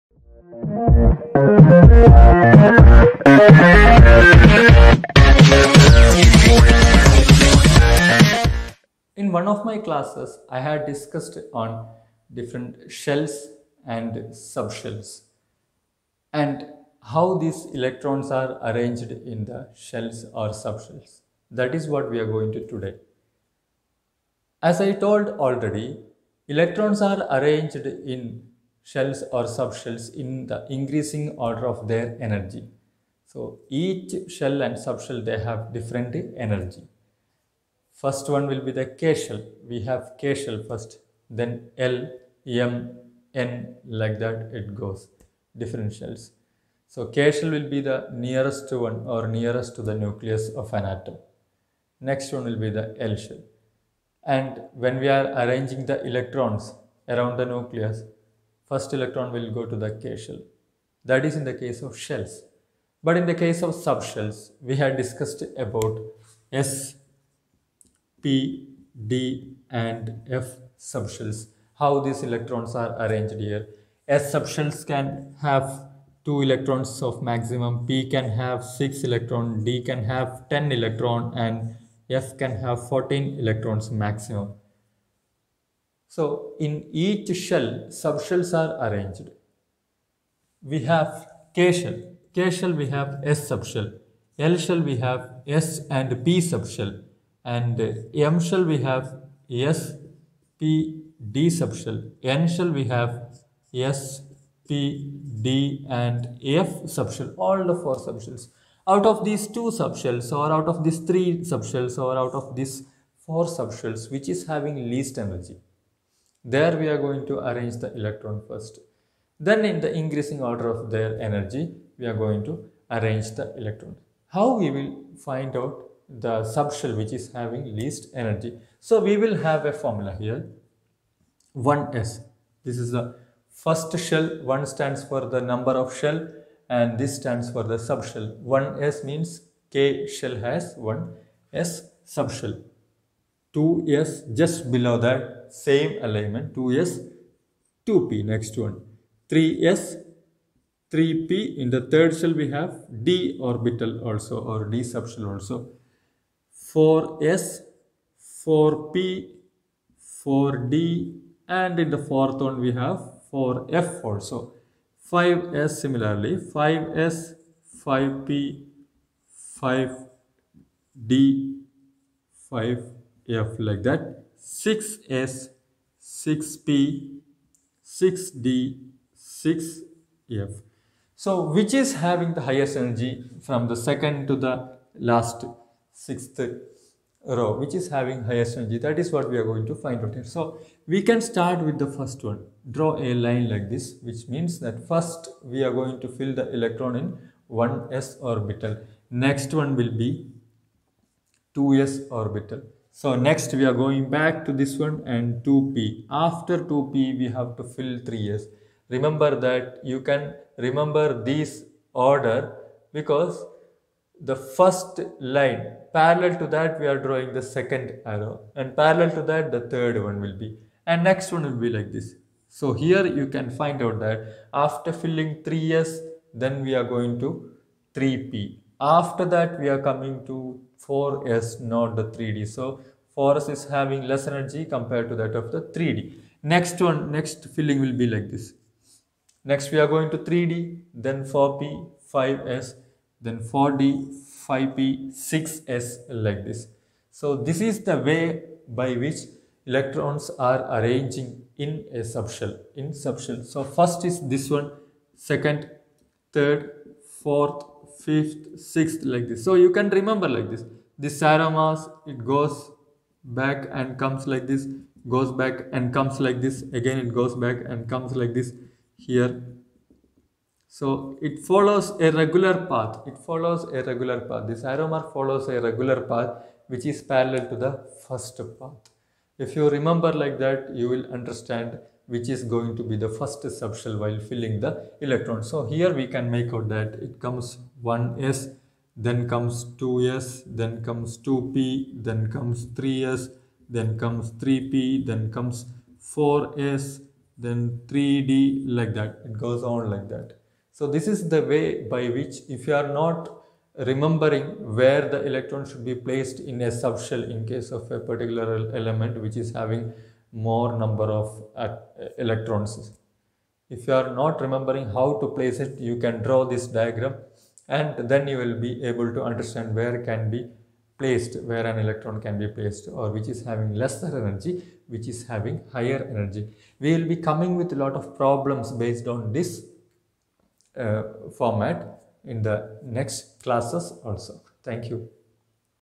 Electronic configuration | Filling of electrons | Energy levels | Orbitals | Sub-shells
Electrons are arranged in the increasing order of the energy levels. Each shell consists of orbitals and each orbital has a specified number of electrons that can be accommodated. Here you can find how to fill electrons in the increasing order of energy levels